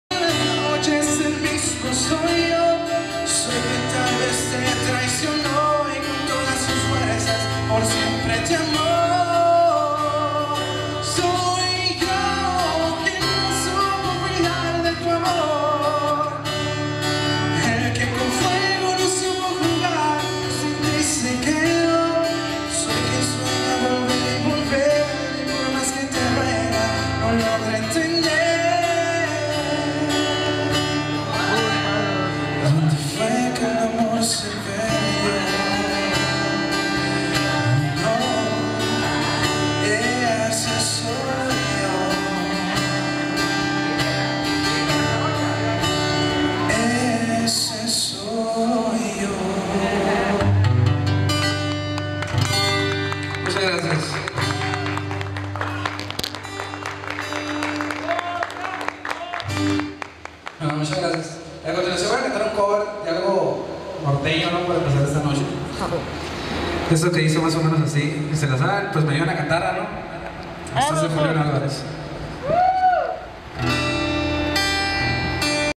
PHIL SPINOZA/TELEVISA MEXICALI/ LOS REYES DE LA FIESTA/FIESTA HOLLOWEEN
PHIL SPINOZA, CANTANTE MEXICALENSE, SOLISTA, MOSTRANDO SU TALENTO EN LA FIESTA DE DISFRACES DE TELEVISA MEXICALI / LOS REYES DE LA FIESTA- ANTRO EL WATEKE, CARRETERA UNION MEXICALI - OCT 2013